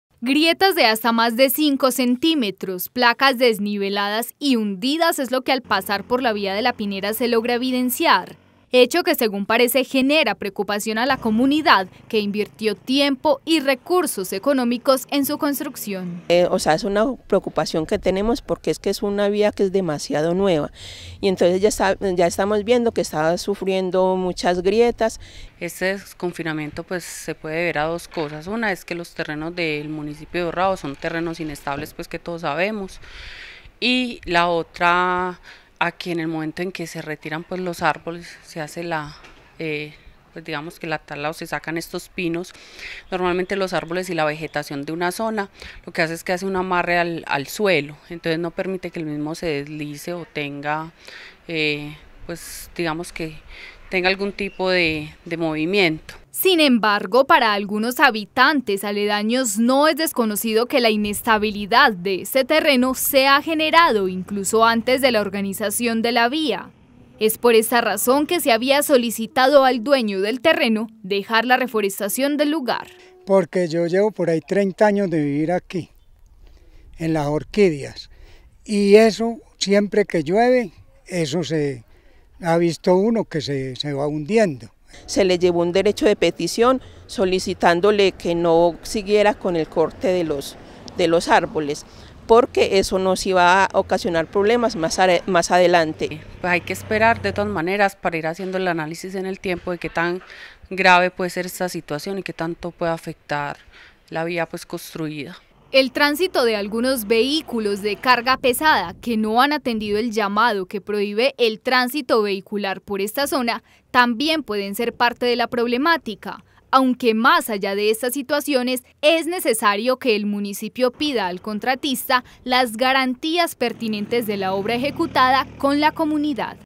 Vía "La Pinera" ya comienza a deteriorarse
Una grave situación se presenta en la vía que conduce de la alcaldía hacia Jaiperá, dos años después de haberla pavimentada presenta problemas estructurales que se pudieron haber dado por la inestabilidad del terreno, el aprovechamiento de la madera que había en este lugar y las lluvias